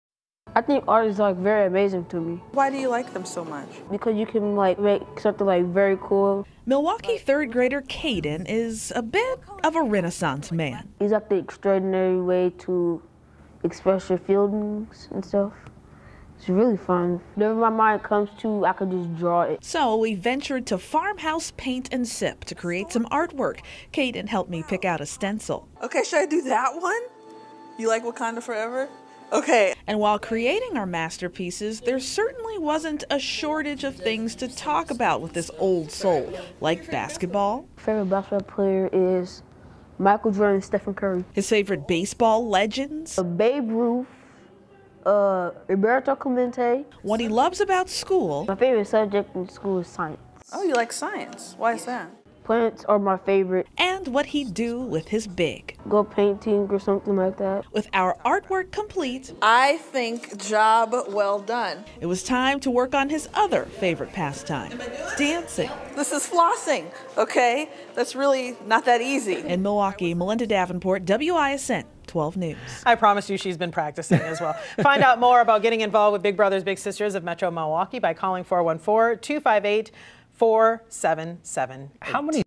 Tuesday's Child Expresses Himself in Art
Third-grader Caden says he can draw whatever he thinks about.